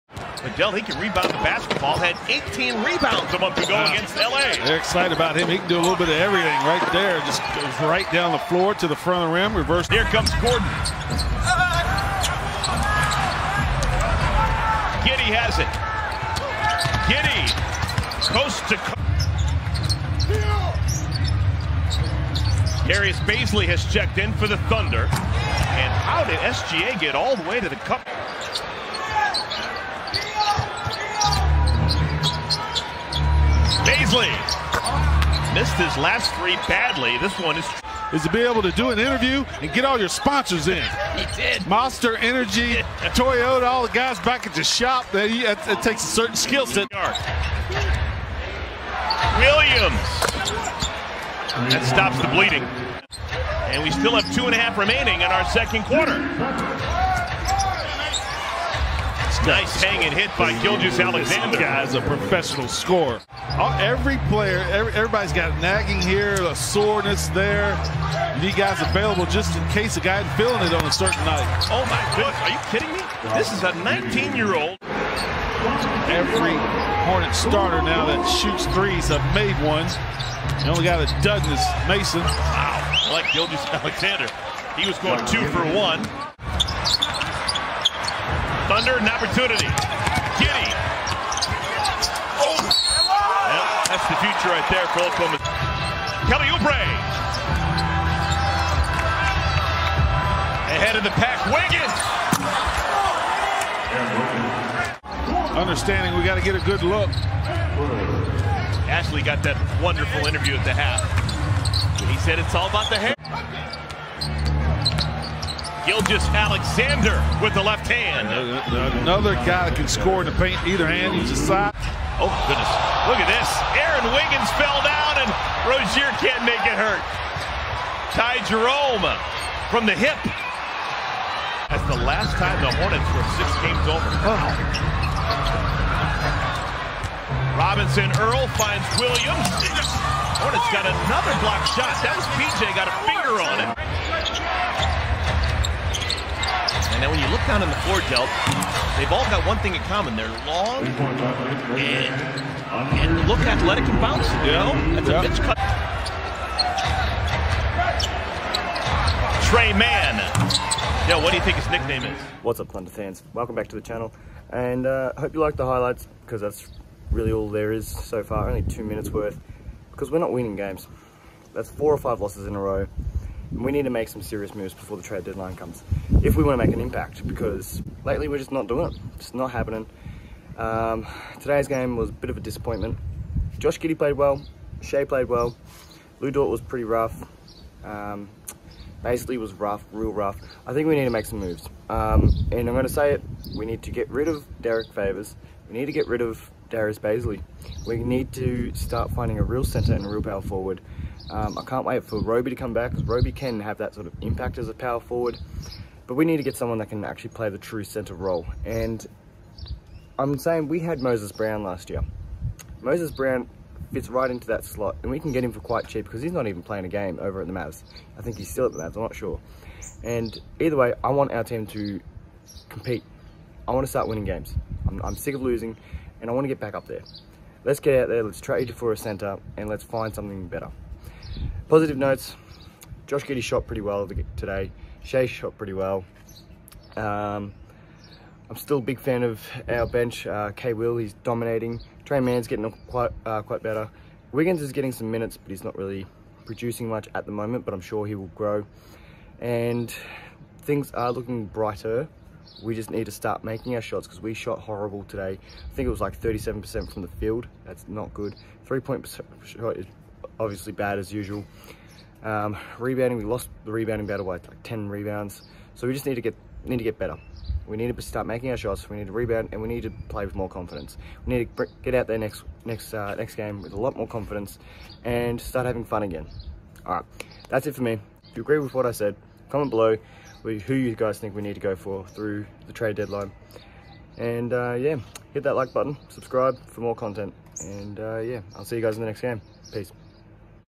OKC Highlights | Trade Deadline Talk | TFDU
Welcome back thunder nation!

Here’s a quick highlight of today loss.
I also talk about what we need to do to improve! That’s if OKC want to improve!